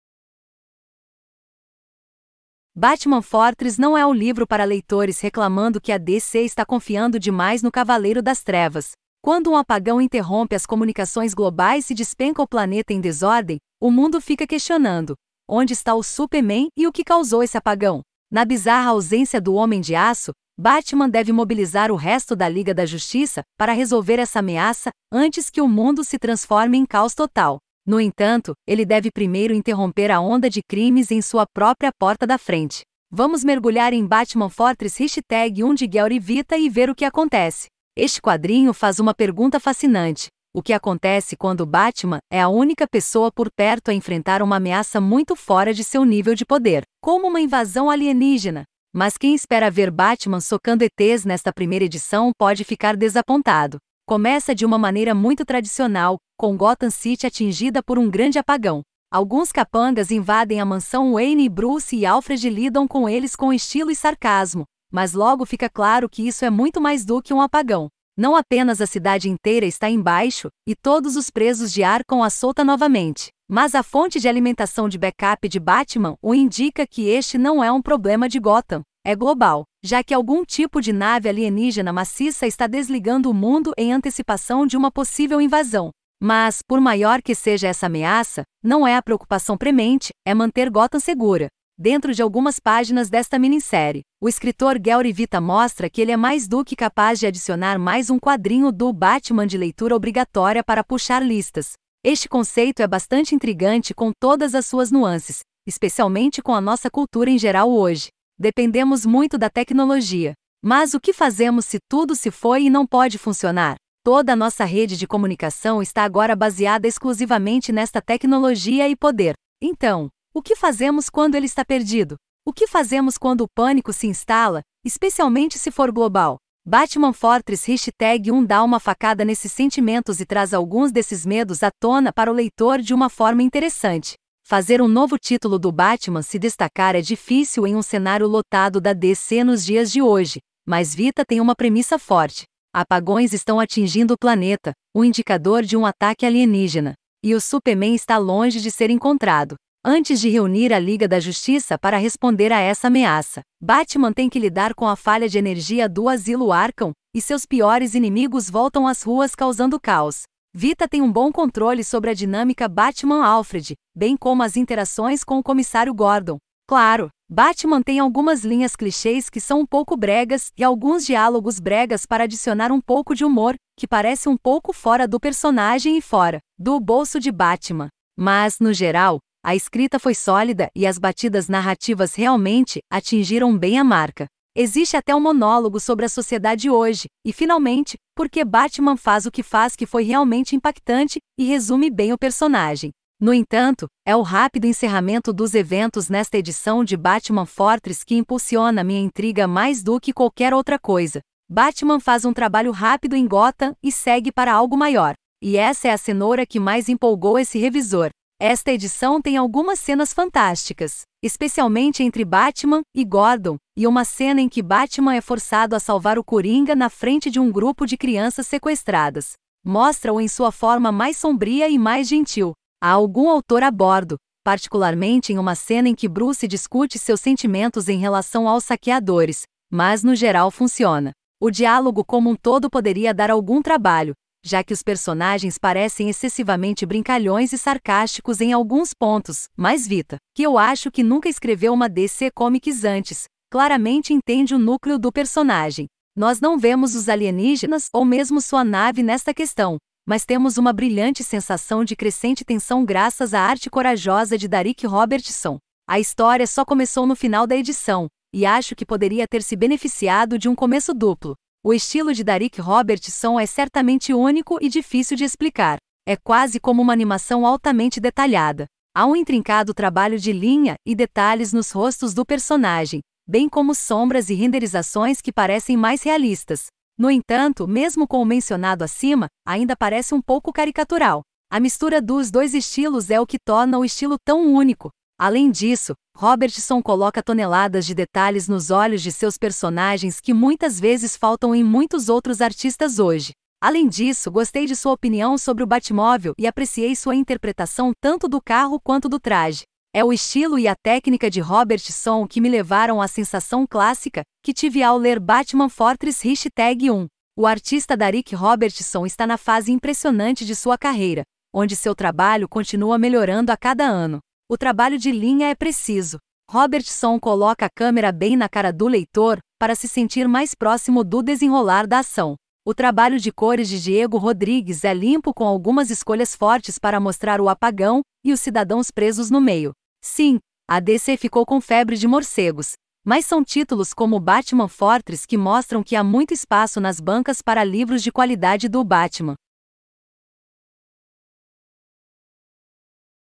Batman Fortress #1 - HQ - Crítica
Batman Fortress não é o livro para leitores reclamando que a DC está confiando demais no Cavaleiro das Trevas. Quando um apagão interrompe as comunicações globais e despenca o planeta em desordem, o mundo fica questionando… onde está o Superman e o que causou esse apagão? Na bizarra ausência do Homem de Aço, BATMAN deve mobilizar o resto da Liga da Justiça para resolver essa ameaça antes que o mundo se transforme em caos total. No entanto, ele deve primeiro interromper a onda de crimes em sua própria porta da frente. Vamos mergulhar em  Batman Fortress #1 de Gary Whitta e ver o que acontece!